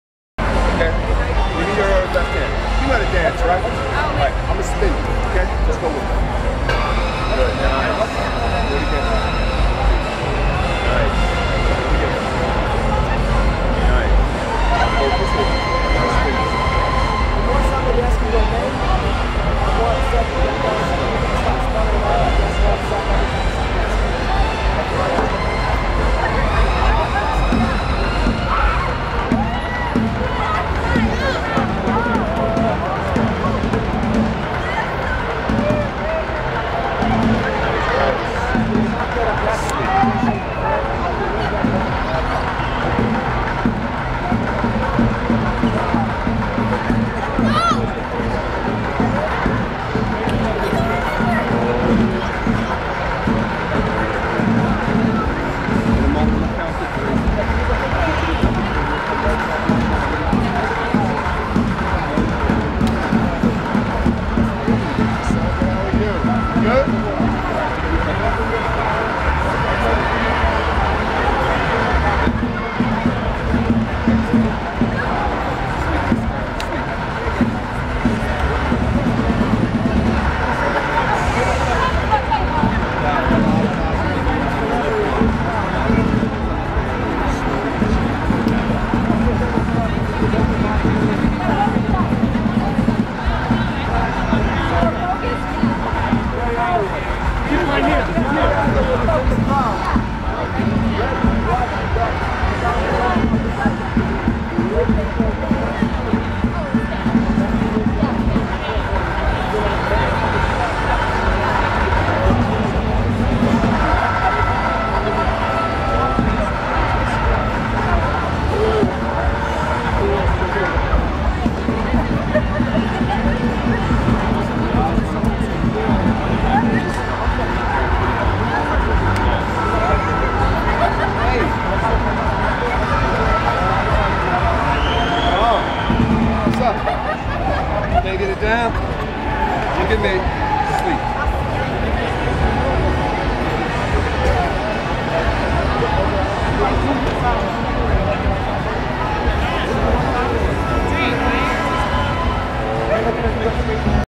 Learn How to Hypnotize Someone
Hypnotizer Derrick Watkins demonstrates how to hypnotize someone in Las Vegas! If you want to learn how to hypnotize people.
Learn Hypnosis NJ  Learn Hypnosis NYC